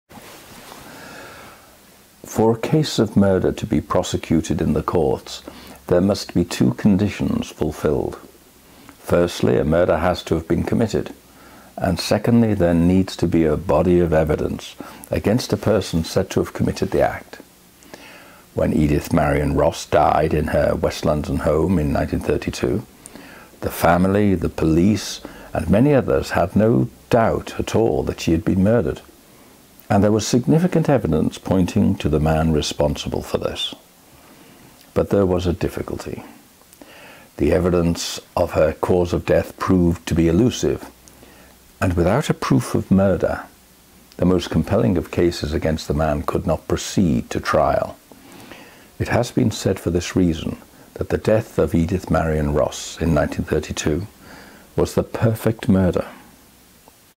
Nailed It In One Take - Mark John Maguire
It's very satisfying to nail a piece to camera in one take. This was a great effort - but there was a slight oversight on my part!!!